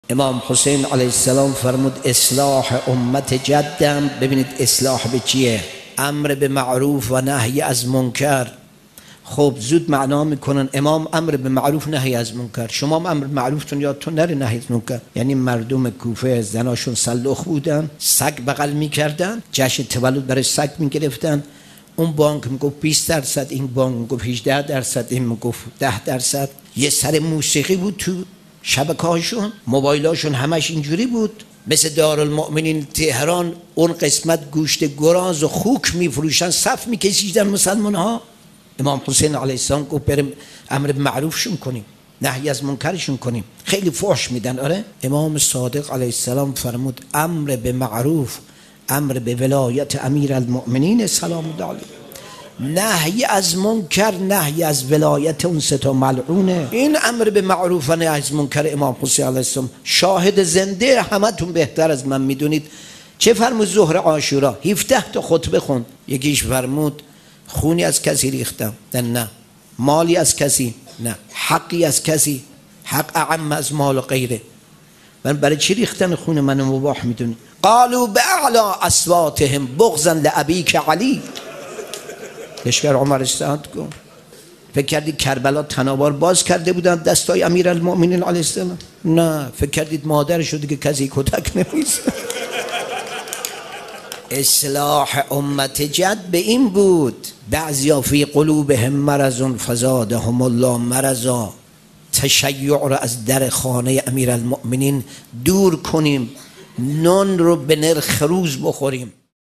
امر به ولایت امیرالمومنین علیه السلام ، امر به معروف حقیقی ست
✅ نکاتی بسیار شنیدنی از معنی واقعی نهی از منکر که هدف قیام حضرت سیدالشهداء علیه‌السلام است
 استاد بندانی نیشابوری

اللهم صل علی محمد وال محمد و عجل فرجهم والعن اعدائهم اجمعین